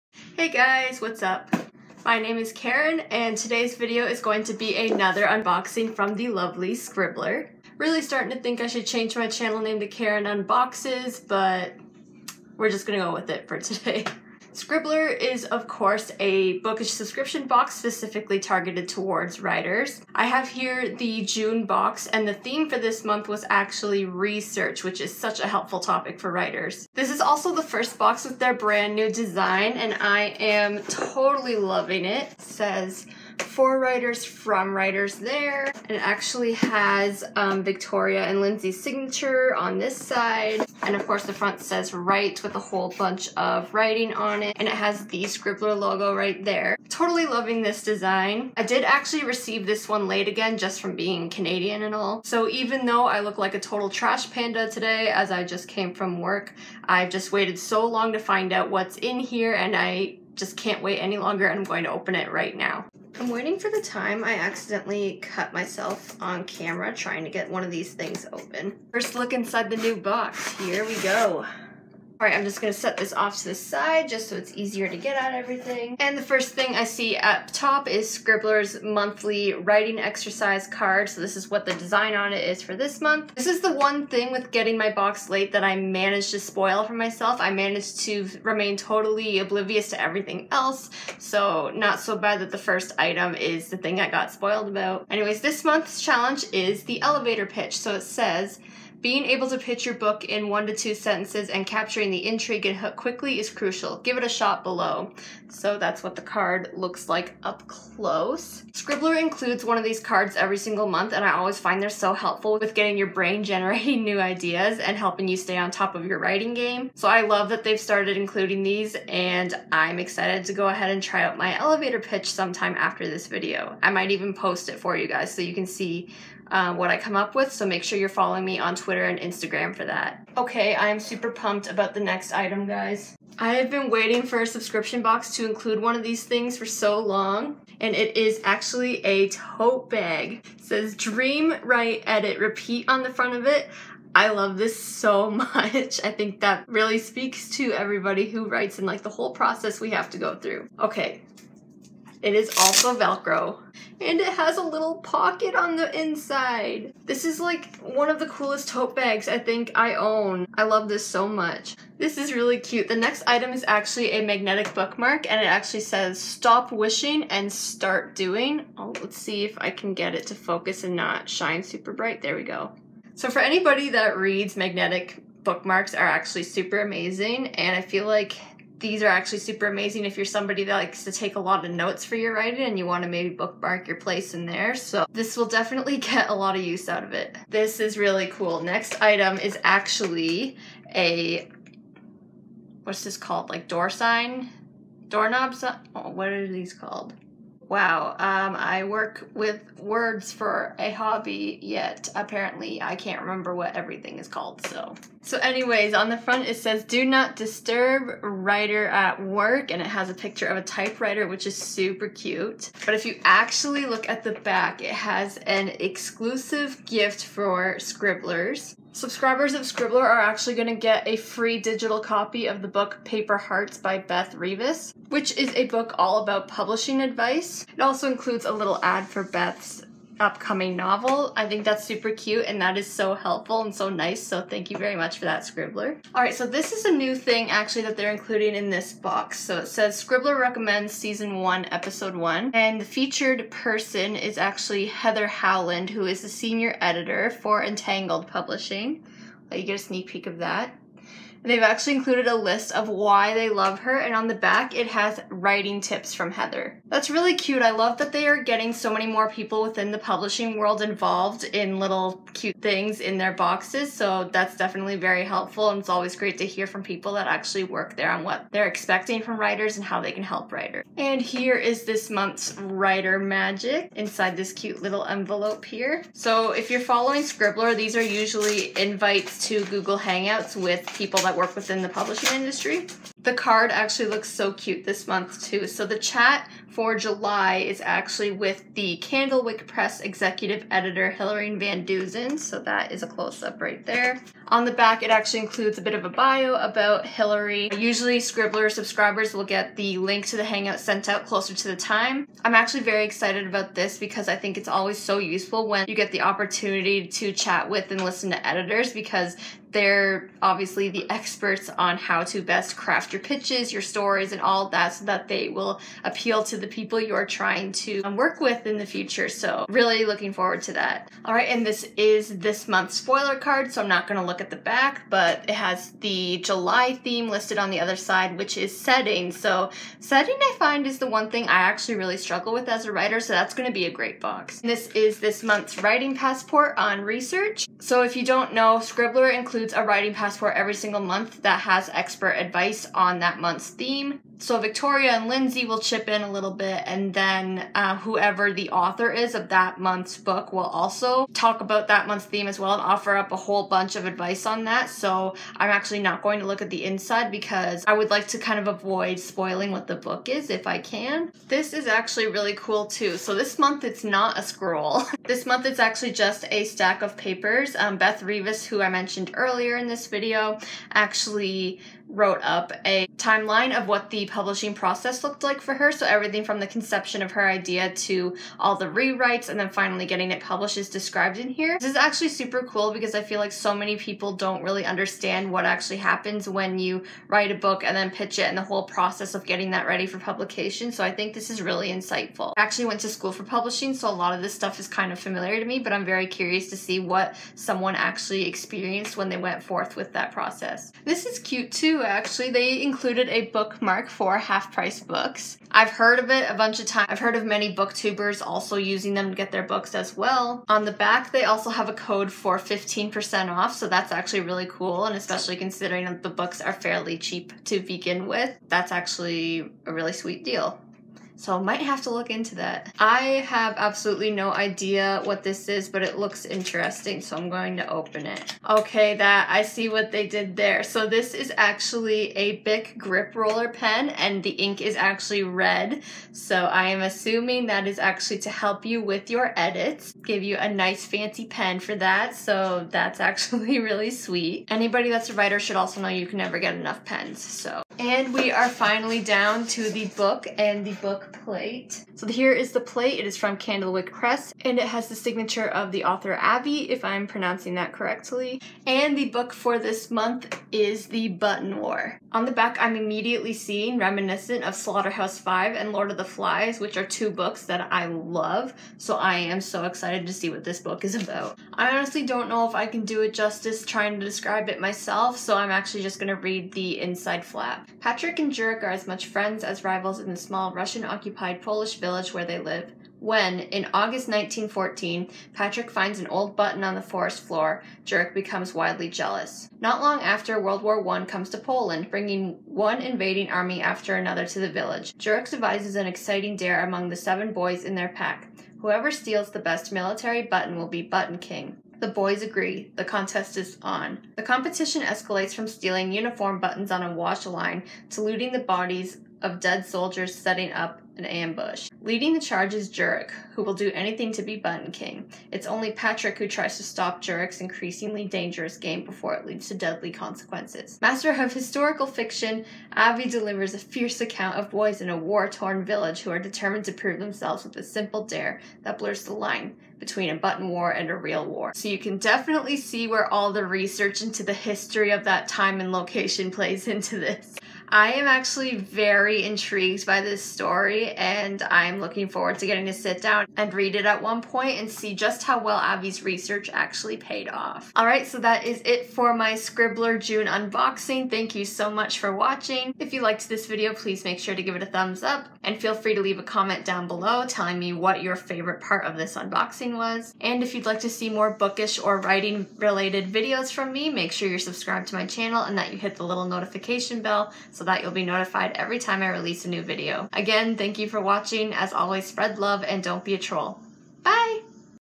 SCRIBBLER JUNE UNBOXING | RESEARCH
After a long wait, my June Scribbler unboxing is finally here!